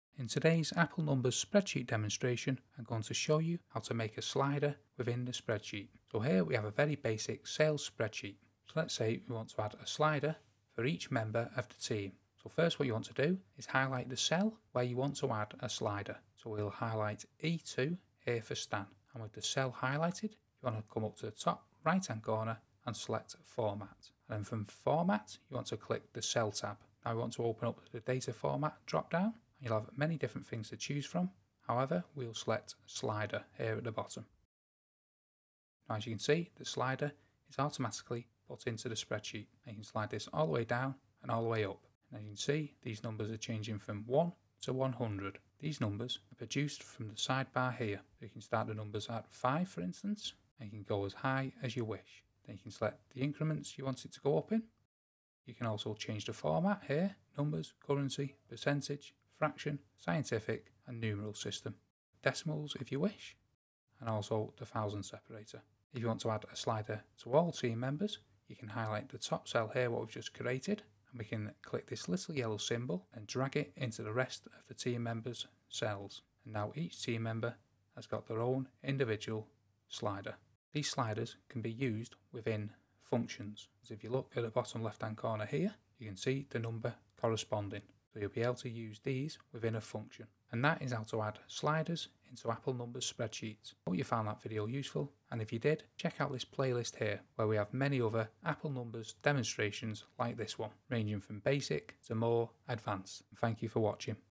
How to Make a Slider in Apple Numbers Spreadsheet
Apple Numbers is a huge app with tons of features. In todays video we will learn How to Make a Slider in Apple Numbers Spreadsheet. We have many of Apple Numbers guides for both beginners and more experienced users.